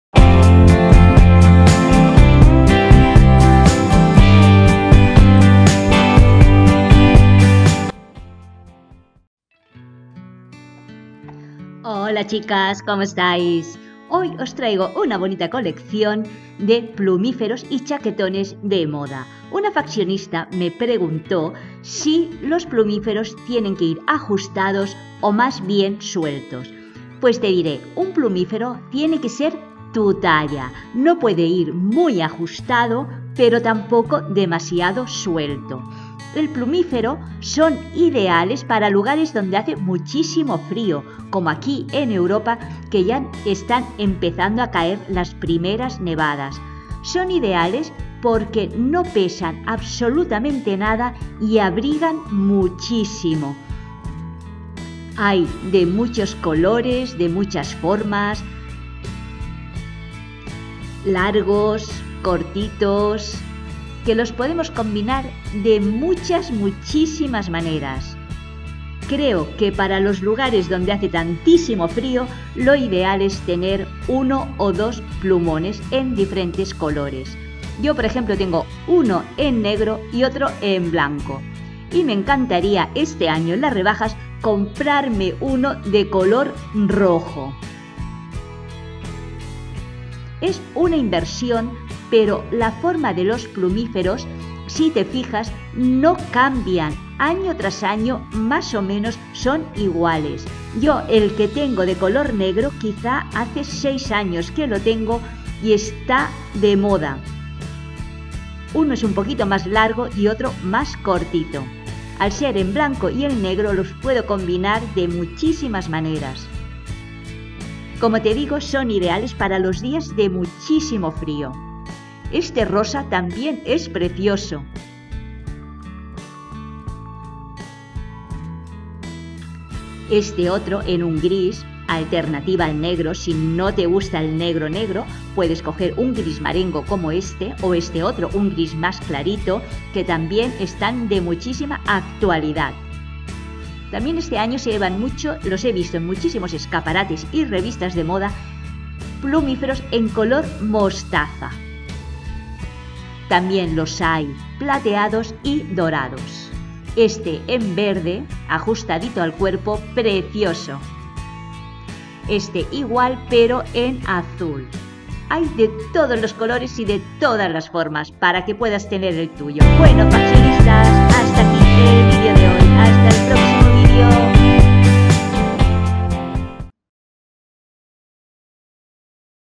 PLUMIFEROS, Abrigos, chaquetas y chaquetones / TENDENCIAS MODA MUJER 2020 / Fashion Love
LOS PLUMIFEROS ESTÁN EN TENDENCIAS DE MODA MUJER PARA EL 2020, ESTÁN EN VERSIÓN CHAQUETA Y EN VERSIÓN CHAQUETONES, ES DECIR LARGOS Y CORTOS, PODEMOS ELEGIR COMO MÁS NOS GUSTEN Y MEJOR NOS SIENTEN. Fashion Love